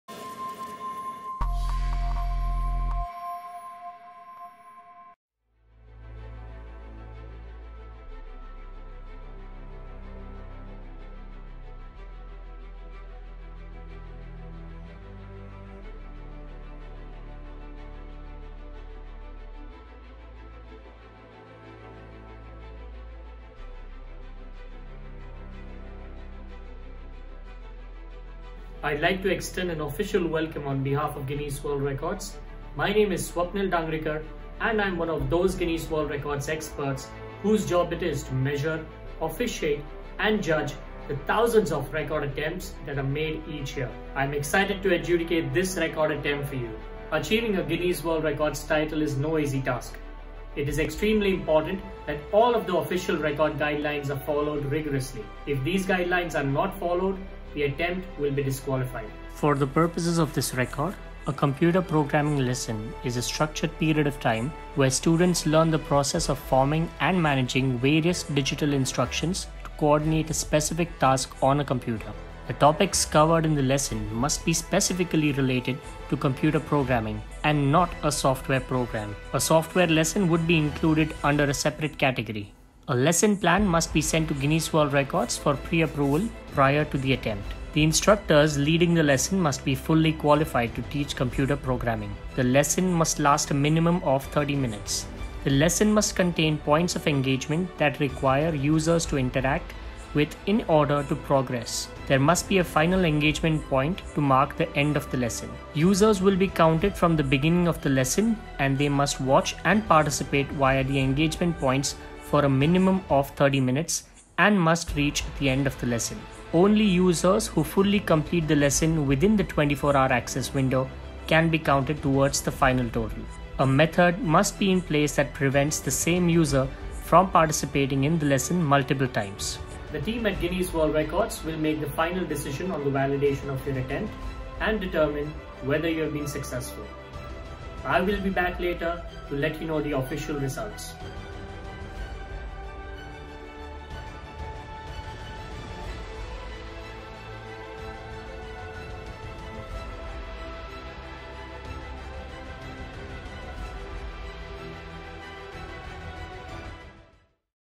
Official Guinness World Record Video | AIforIndia Virtual event - GUVI | BUDDI.AI
Official Video from 𝐆𝐮𝐢𝐧𝐧𝐞𝐬𝐬 𝐖𝐨𝐫𝐥𝐝 𝐑𝐞𝐜𝐨𝐫𝐝 Team for GUVI

We are organising a grand event on AIforIndia 1.0" in association with AICTE, co-organised by BUDDI.AI

The event is registered with Guinness World Record in terms of having more participants on a single virtual event.

The best part is anyone who has an interest in technologies can attend.

𝐖𝐡𝐚𝐭 𝐚𝐫𝐞 𝐭𝐡𝐞 𝐛𝐞𝐧𝐞𝐟𝐢𝐭𝐬 𝐟𝐨𝐫 𝐲𝐨𝐮?
✓ Free access to our Python course
✓ Participant certificate from GUVI on behalf of Guinness World Record
✓ Learn to build a Face Recognition app using Python & get a Certificate for the same

The grand event is scheduled for April 24th, Where you will learn to build a Face Recognition app using Python.